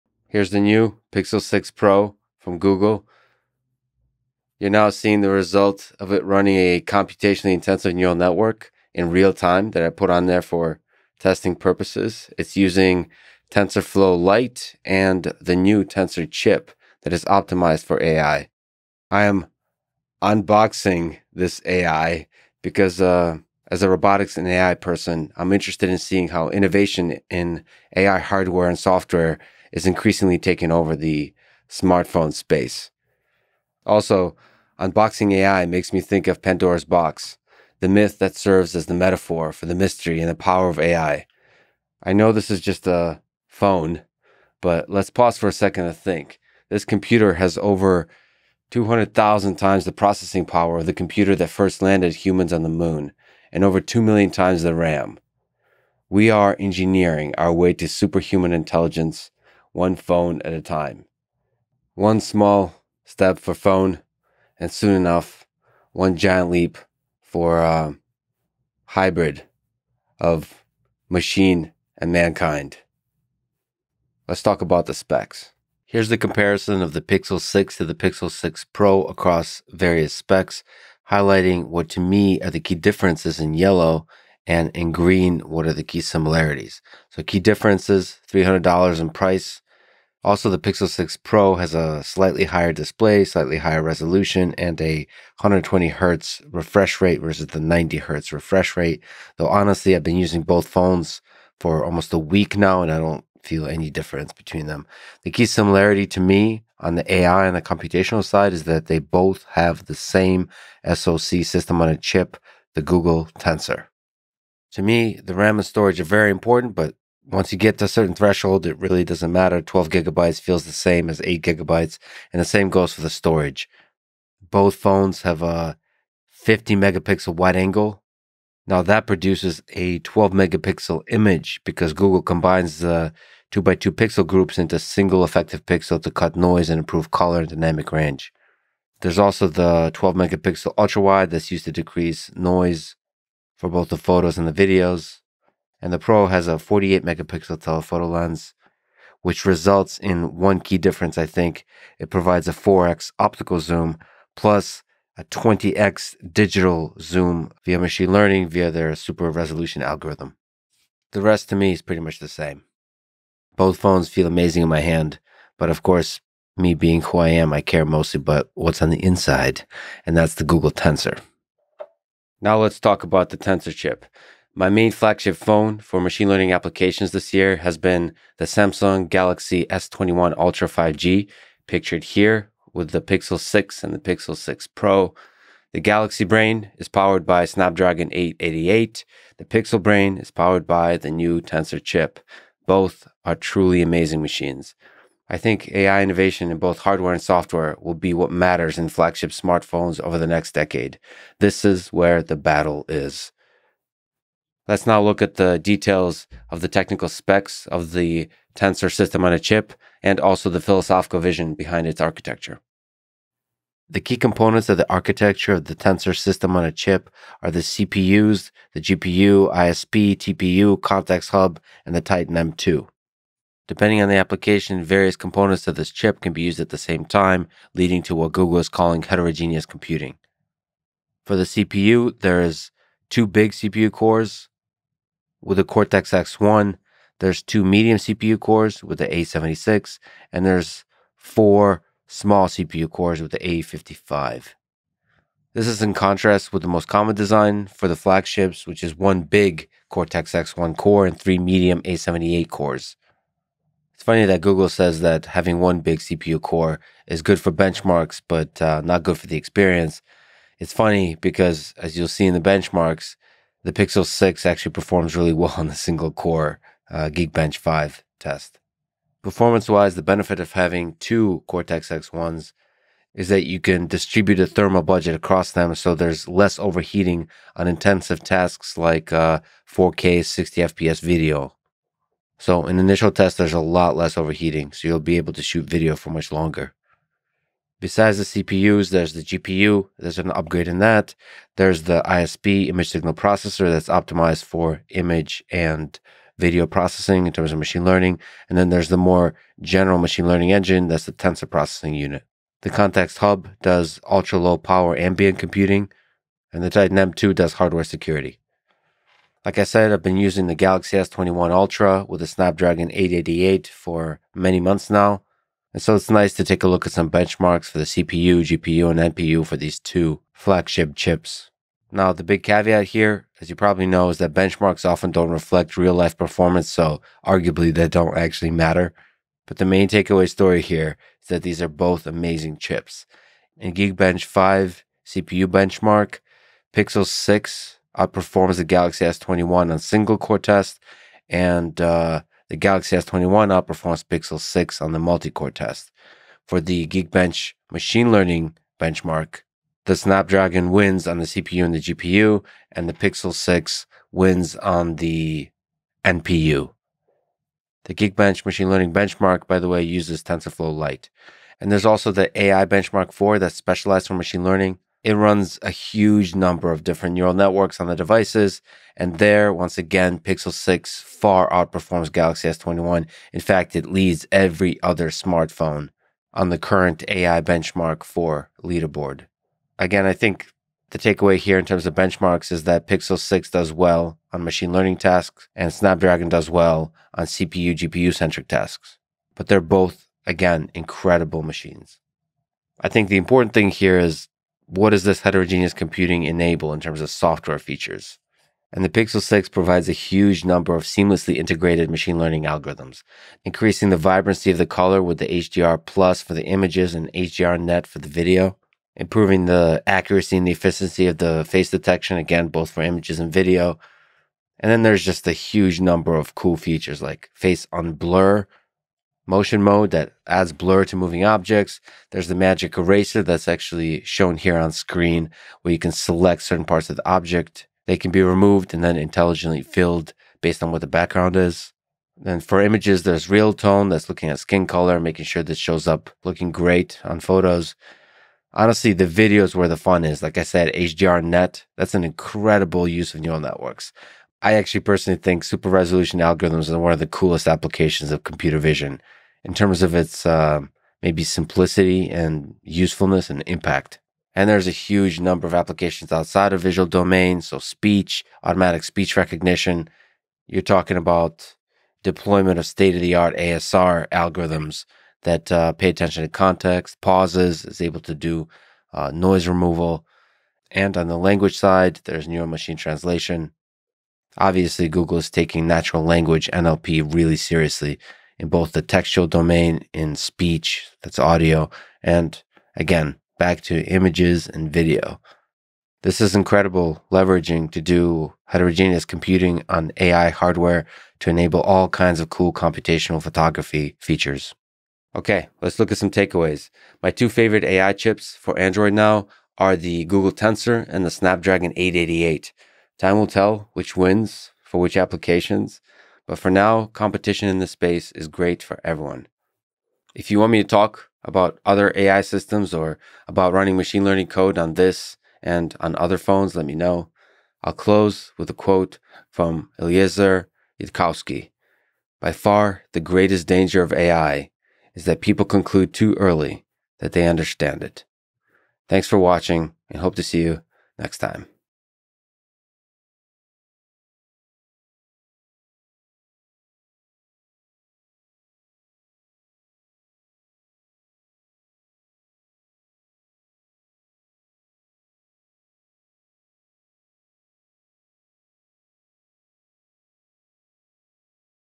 Pixel 6 AI explained | Lex Fridman
A demo and detailed look at the Pixel 6 and Pixel 6 Pro with a focus on AI and the Tensor chip.

OUTLINE:
0:00 - ML demo on phone
1:13 - Specs
2:52 - Tensor - AI hardware
5:09 - Benchmarks
6:32 - AI software
8:14 - Conclusion